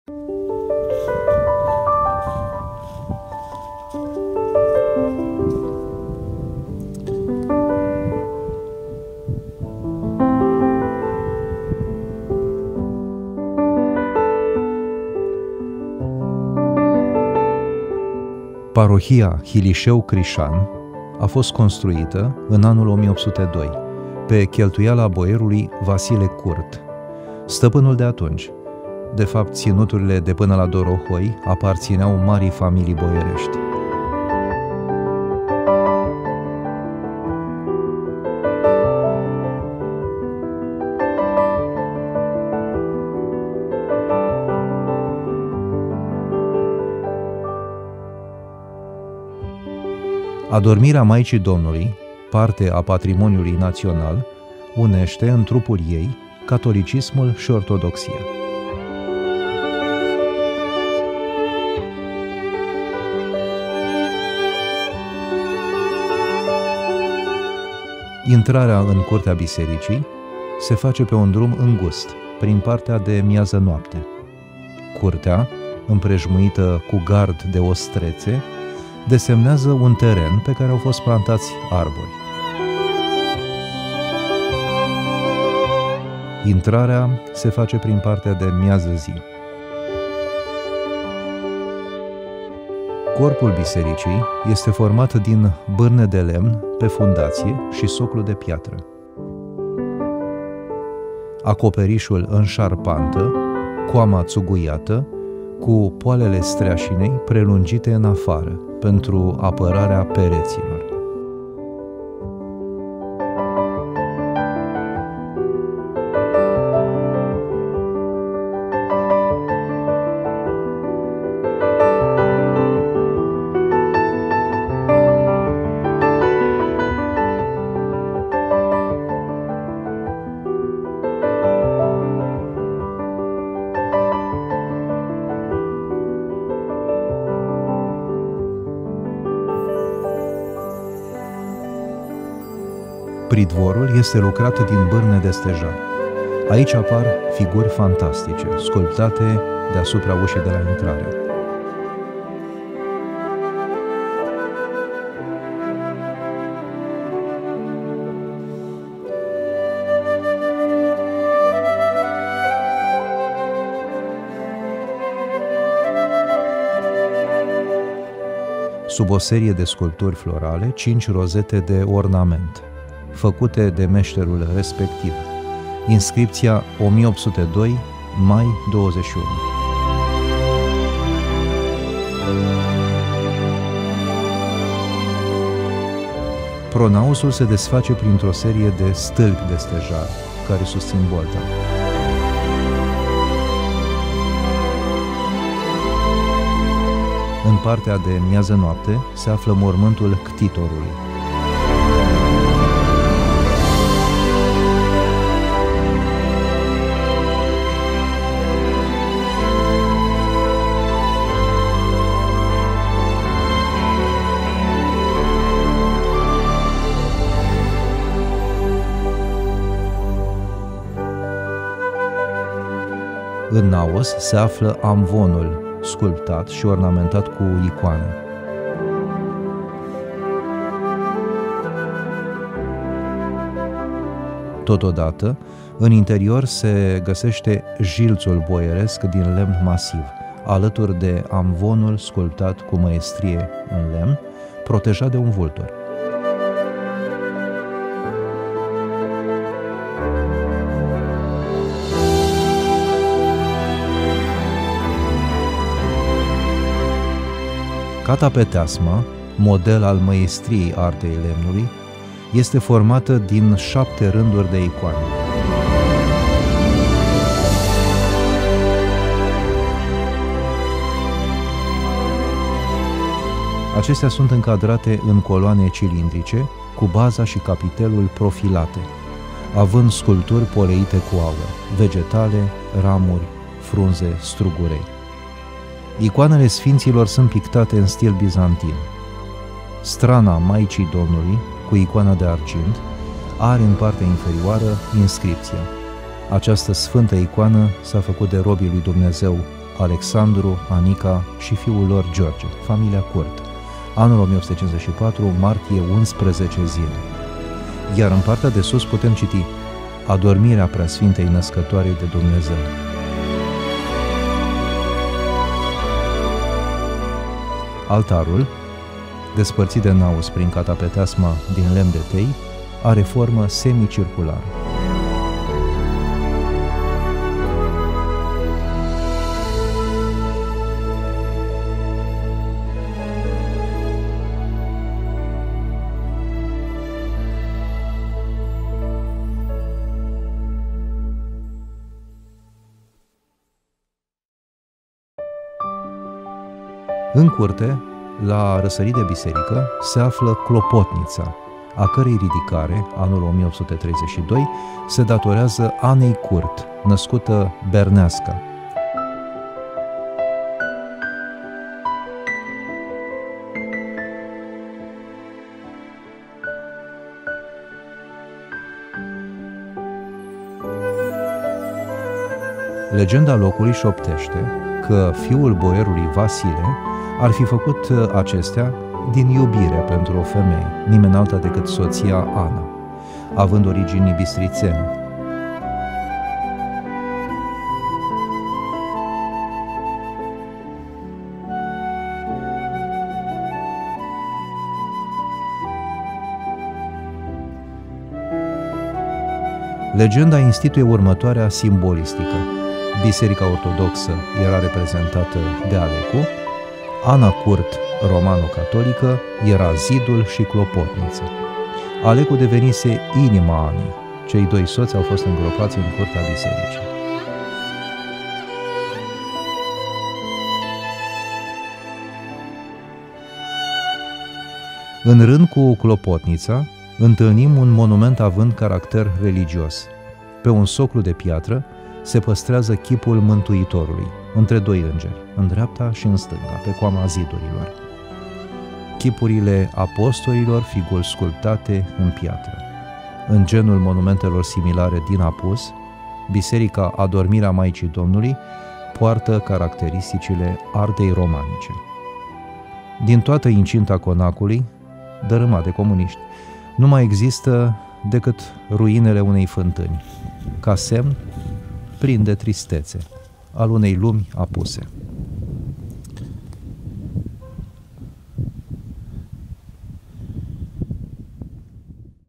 FILM Biserica de lemn din Hilișeu Crișan
O biserică din lemn întâlnită, în câmp, între Hilișeu-Horia și Hilișeu-Crișan, la 2 km distanță de fiecare sat, în apropierea pădurii „Călugărița”, la câțiva kilometri de granița cu Ucraina...